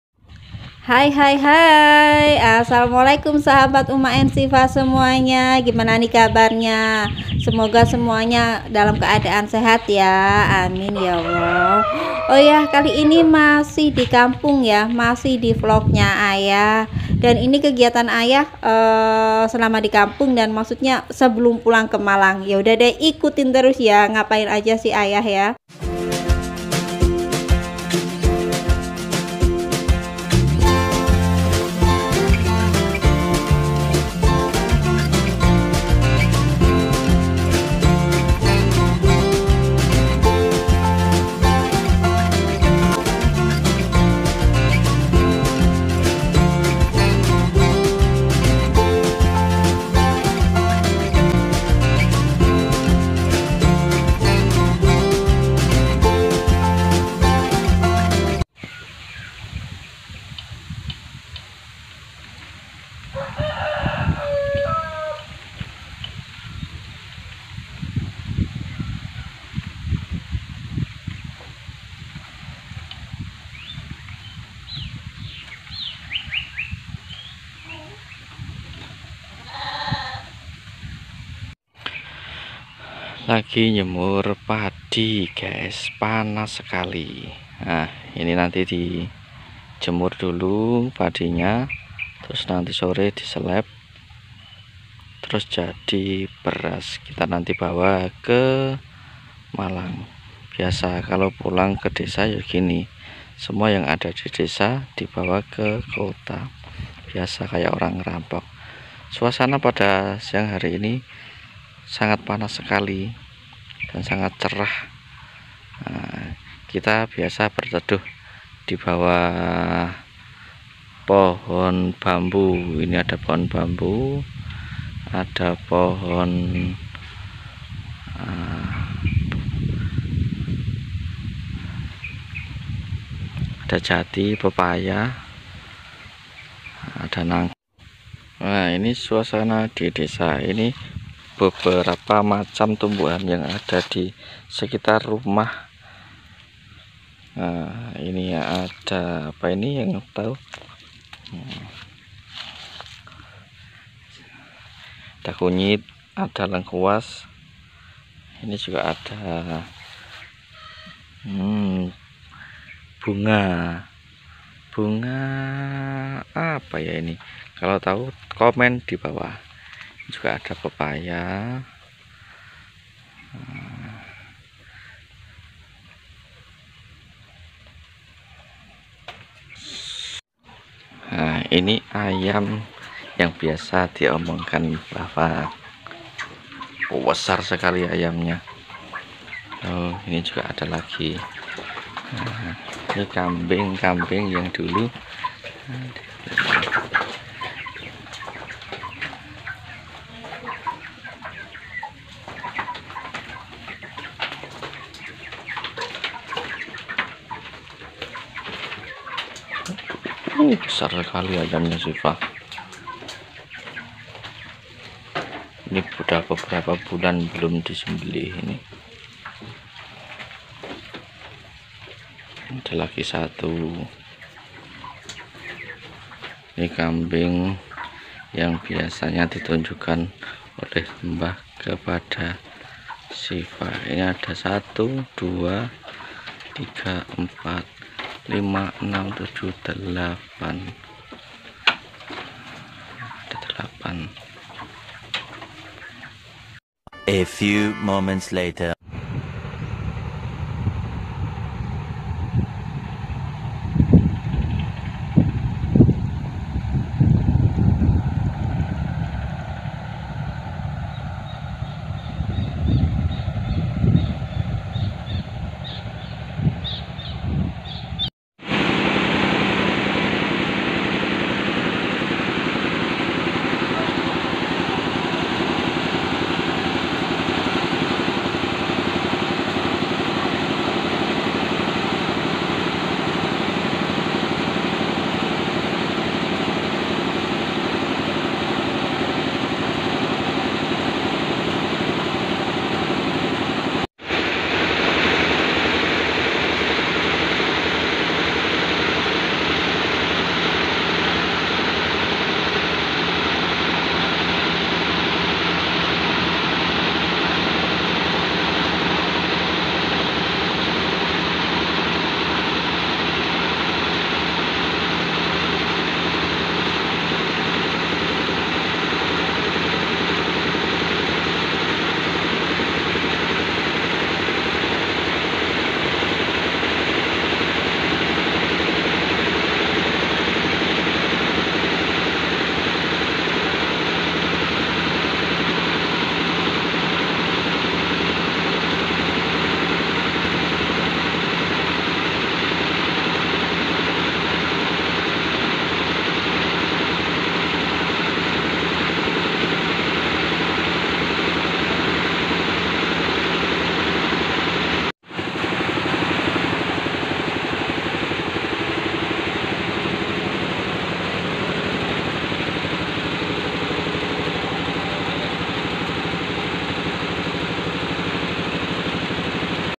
KEGIATAN AYAH SEBELUM PULANG KE MALANG‼️
Masi di vlognya ayah ya teman2, ini kegiatan ayah sebelum pulang kemalang, ya itu jemur pagi dan langsung di giling padinya karna mau di bawa ke malang,